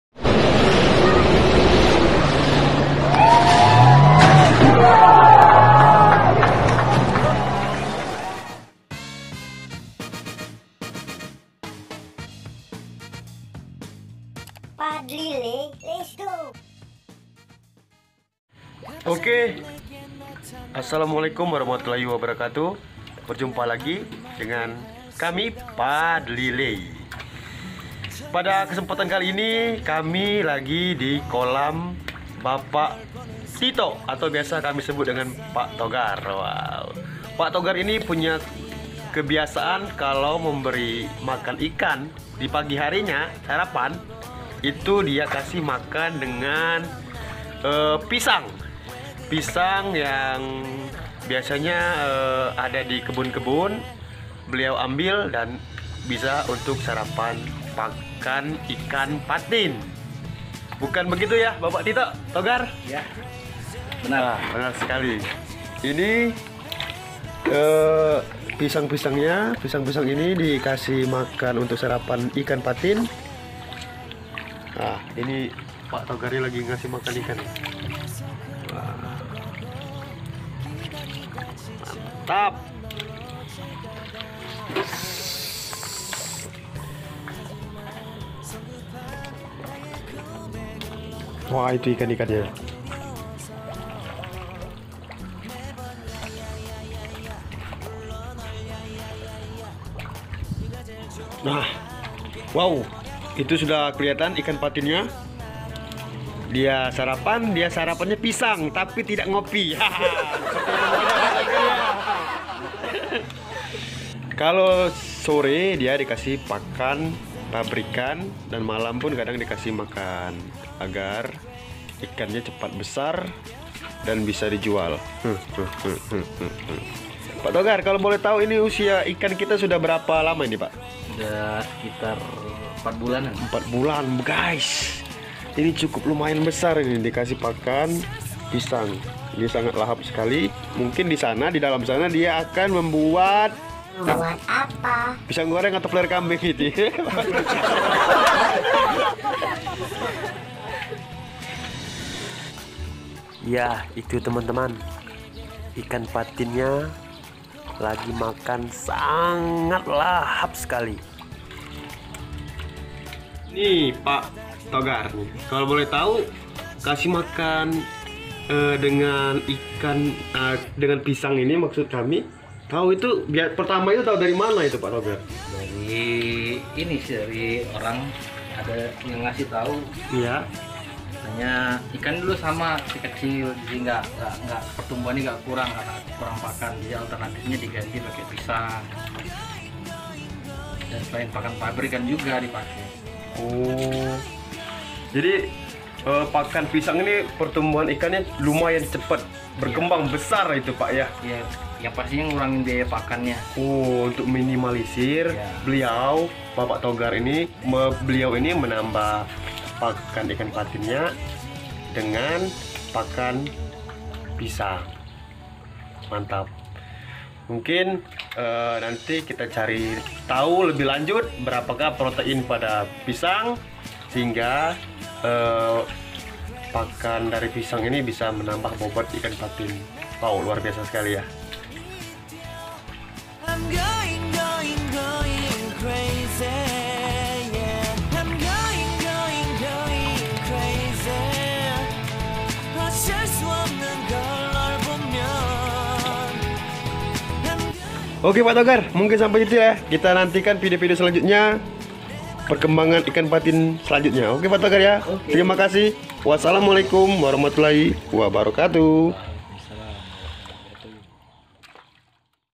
PAKAN TAMBAHAN IKAN PATIN (PISANG)
Pakan tambahan ikan patin di beri pisang sehingga dapat meminimalisir biasa kos pakan.
pakan tambahan ikan patin ini selain pak togar juga sudah ada yang melakukannya atau mencobanya dan hasilnya baik.
tinggal kedepan kita cari tau berapa protein dan dampak dari pakan tambahan ikan patin berupa pisang ini.

terimakasih
jangan lupa makan ikan